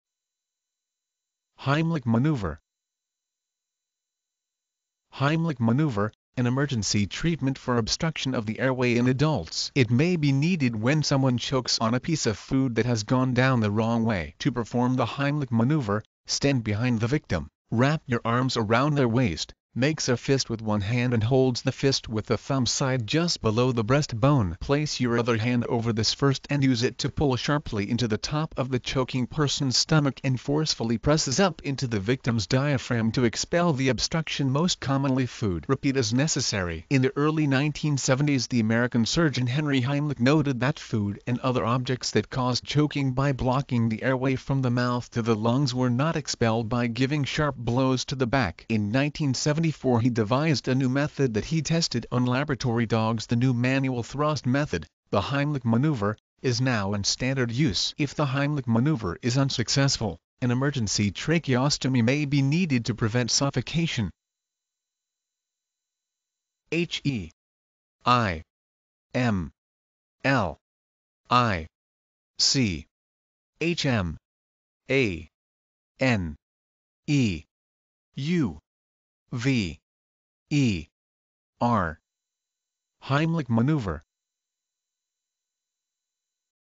Heimlich maneuver - Medical Definition and Pronunciation
Title: Heimlich maneuver

Heimlich maneuver: An emergency treatment for obstruction of the airway in adults. It may be needed when someone chokes on a piece of food that has gone down the wrong way. To perform the Heimlich maneuver, stand behind the victim, wrap your arms around their waist, makes a fist with one hand and holds the fist with the thumb side just below the breast bone. Place your other hand over this first and use it to pull sharply into the top of the choking person's stomach and forcefully presses up into the victim's diaphragm to expel the obstruction (most commonly food). Repeat as necessary. In the early 1970s the American surgeon Henry Heimlich noted that food and other objects that caused choking by blocking the airway from the mouth to the lungs were not expelled by giving sharp blows to the back. In 1974 he devised a new method that he tested on laboratory dogs.The new manual thrust method -- the Heimlich maneuver -- is now in standard use. If the Heimlich maneuver is unsuccessful, an emergency tracheostomy may be needed to prevent suffocation.